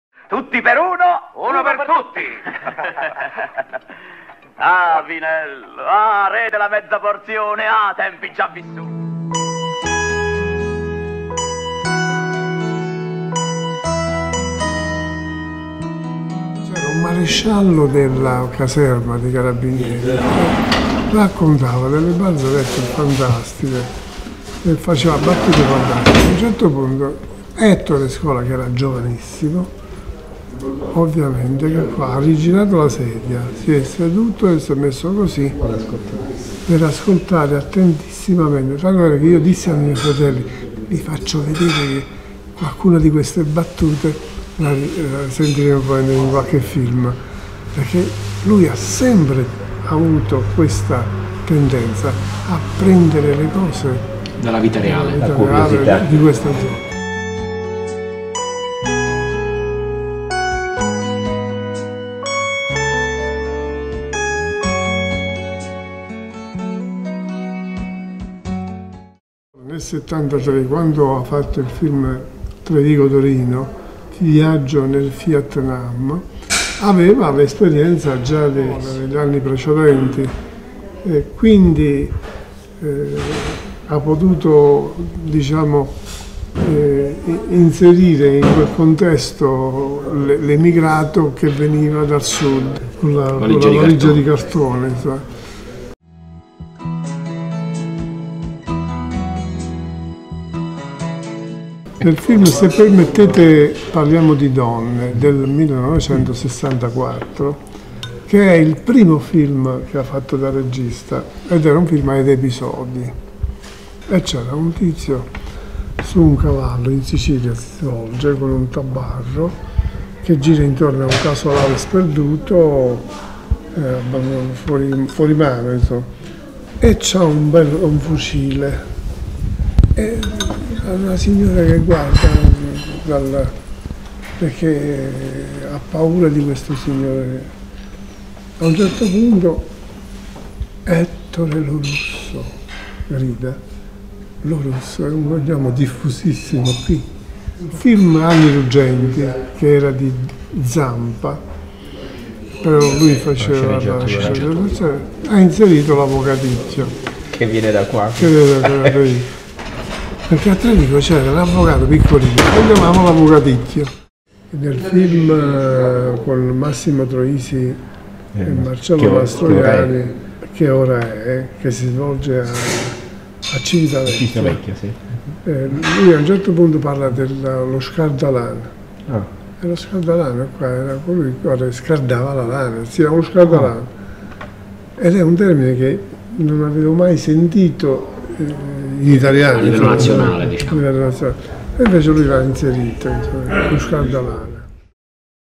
Franco Pagliarulo del ristorante da Minicuccio descrive Trevico nei film di Scola
Ettore Scola andava spesso a Trevico per andare a trovare i suoi genitori. Si fermava a mangiare e a dormire nell'albergo ristorante "Da Minicuccio" a Vallesaccarda, diventando amico con il suo proprietario Franco Pagliarulo. Il professore ha visto tutti i film del Maestro e ha trovato tracce dell'Irpinia in molte pellicole. Seduti a tavola come nella cena dal "Re della mezza porzione" del film "C'eravamo tanto amati", il prof. Pagliarulo ricorda Ettore Scola in compagnia di Marco Lombardi, ideatore della Cinegustologia e Roberto Conte, direttore scientifico del primo Festival della Cinegustologia che si svolge a Trevico, Vallesaccarda e Zungoli.